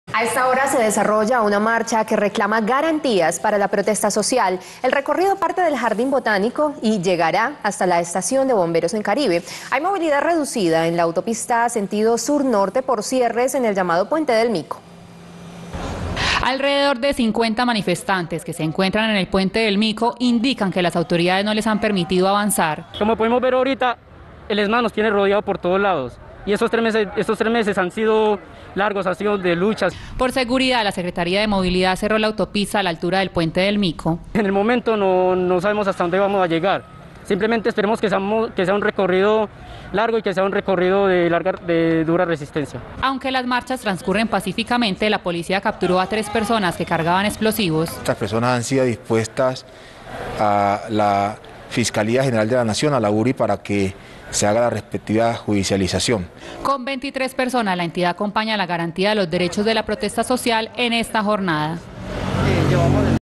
Marcha por garantías y respeto durante la protesta social
El recorrido parte del Jardín Botánico y llegará hasta la Estación de Bomberos en Caribe.